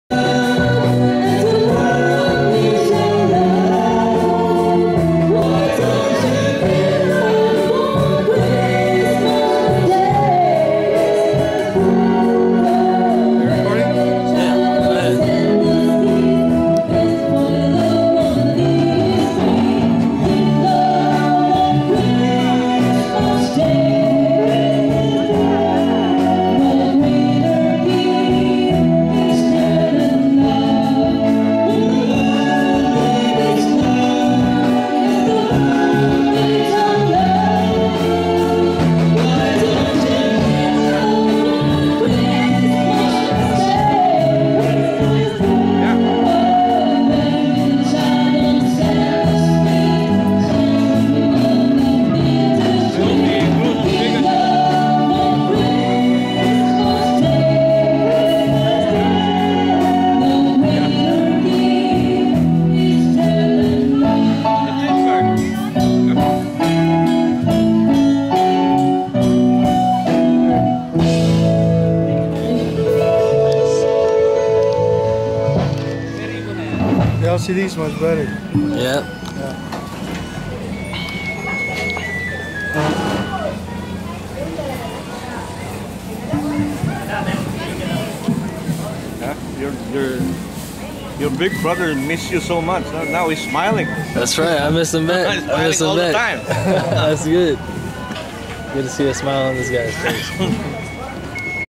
M2U00015.MPG
a live band playing and singing at the Christmas party in Cainta.  Most of the things are set up right on the street next to tita Vicky's water station